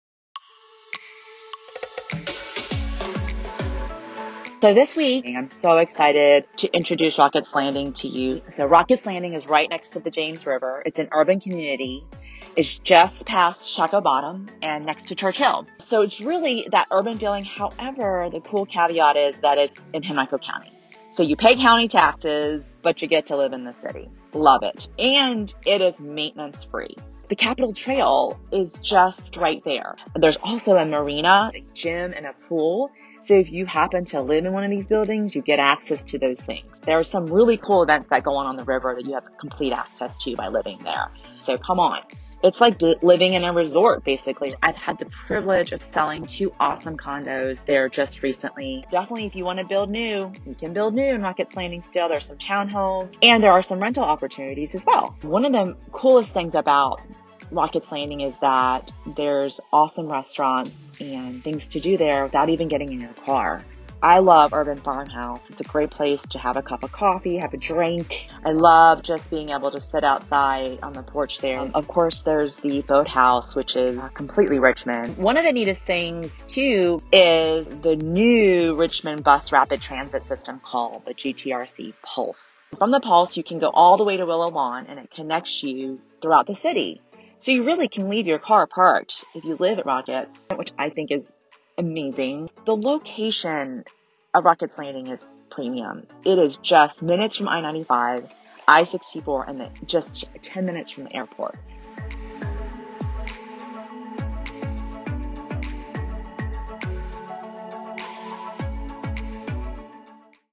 Do You Know RVA? Rocketts Landing
Come check out Rocketts Landing and see how Urban living is in RVA. I recently have sold two condos here and it's a hot neighborhood growing with new construction